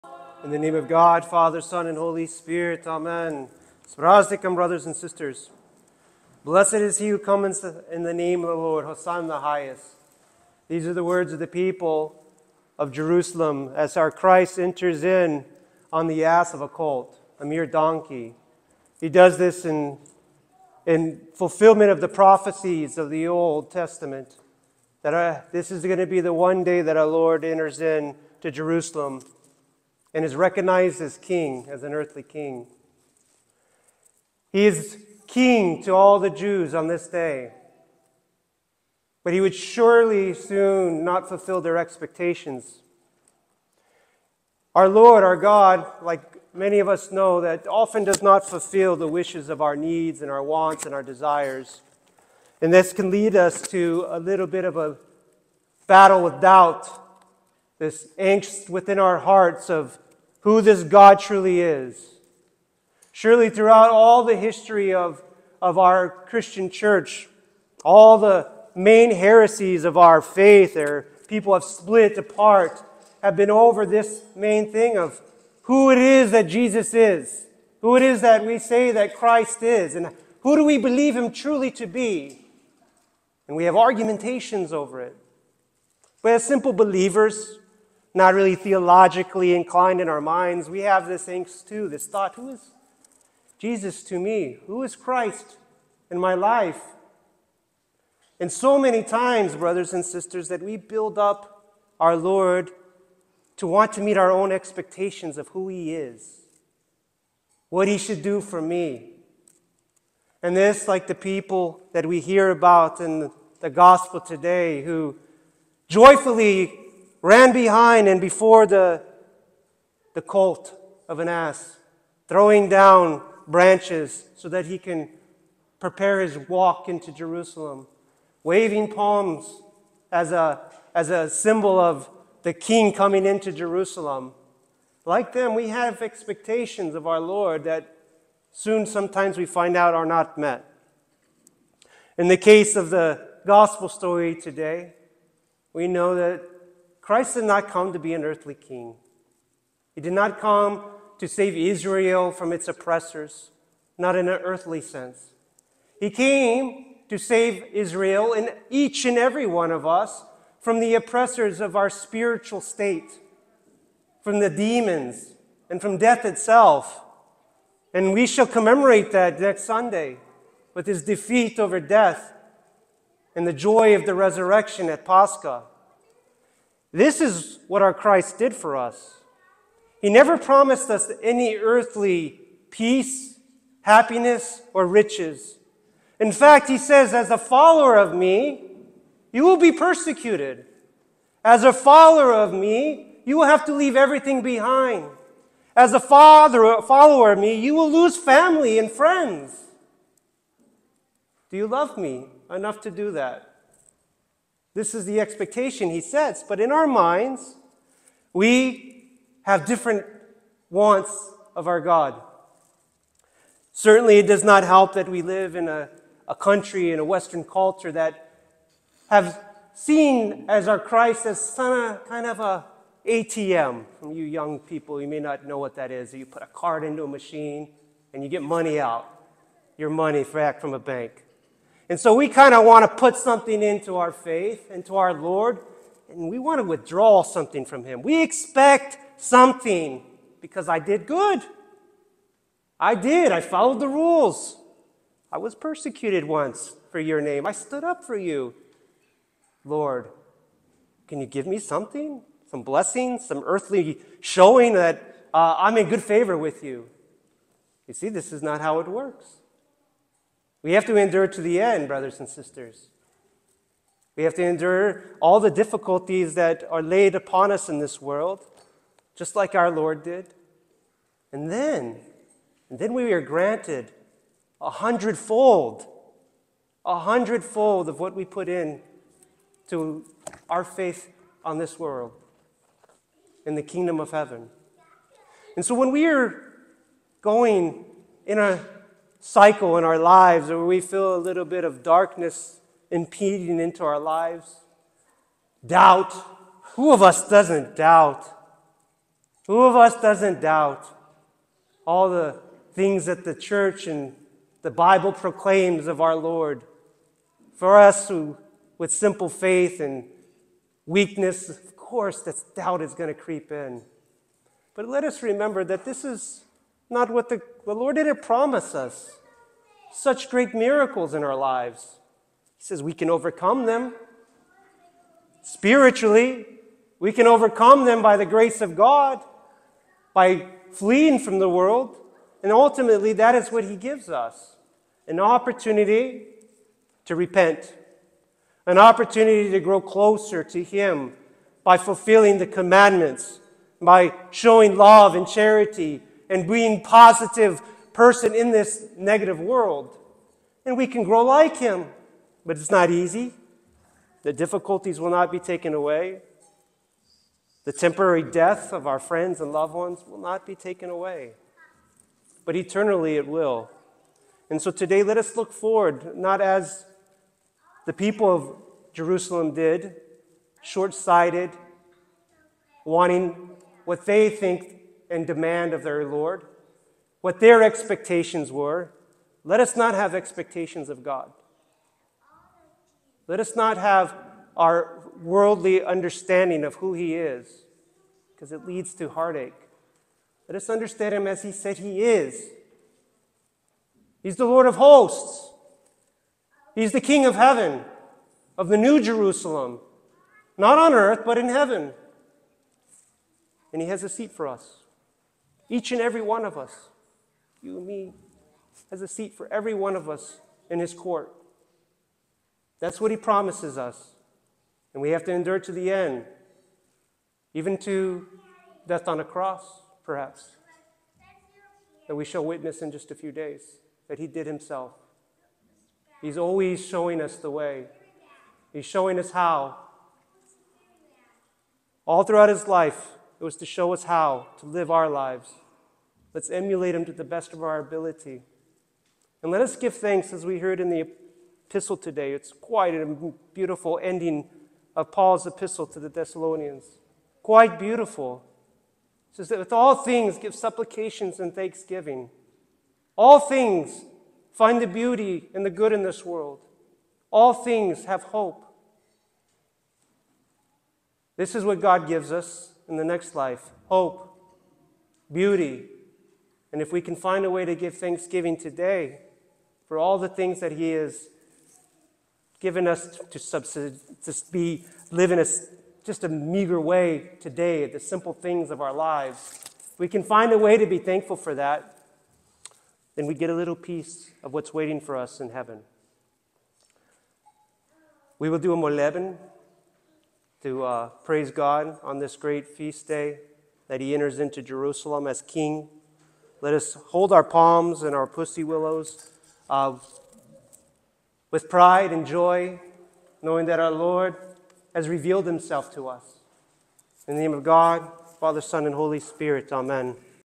The false expectations of Christ
Sermon on the feast of Palm Sunday